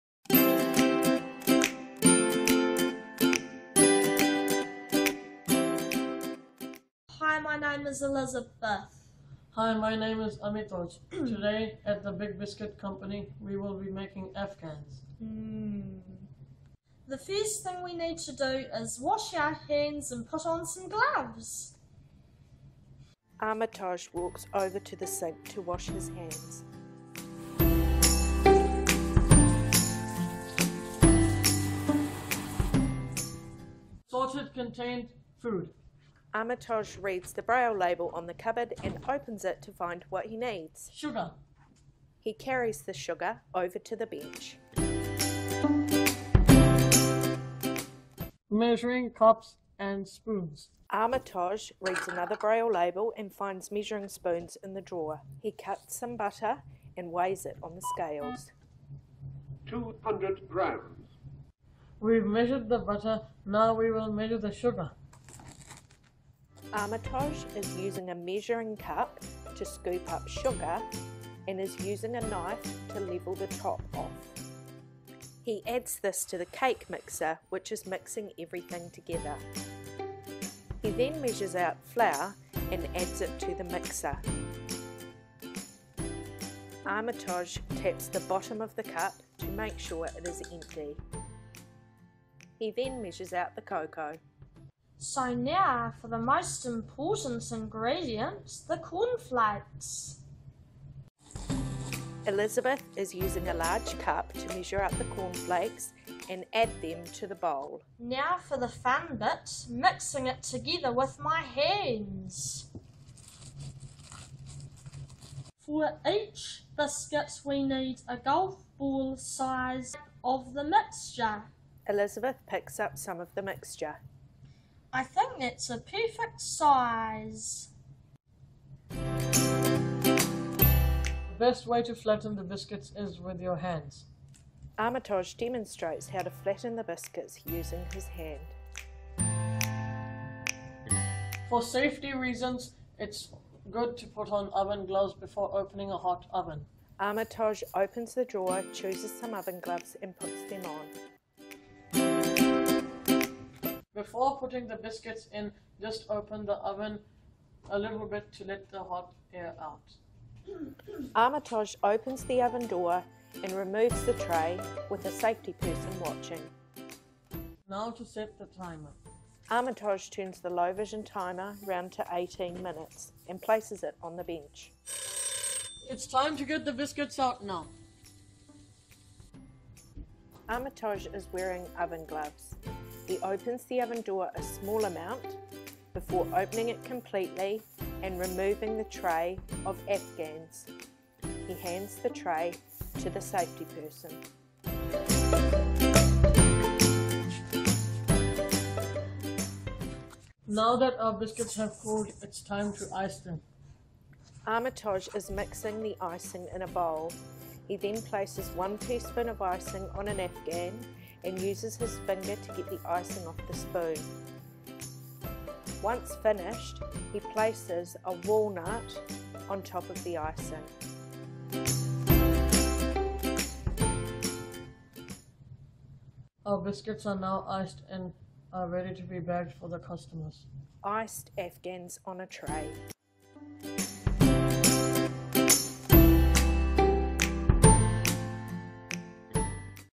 Blind and Low Vision Education Network NZ - Big Biscuits
2016 School Documentary Entrant - Focus on Ability Short Film Festival.

Elizabeth and Amitoj are blind and have no light perception.  In this short documentary they demonstrate the process of baking a batch of biscuits – from organizing the equipment and ingredients, measuring, mixing and forming the dough and finally baking a batch of Chocolate Afghans for their Big Biscuit Company. The kitchen environment has been set up for blind users and has braille labels, ease of access and adapted equipment. The methods and techniques used have been specifically taught and adapted for each student to develop their independence.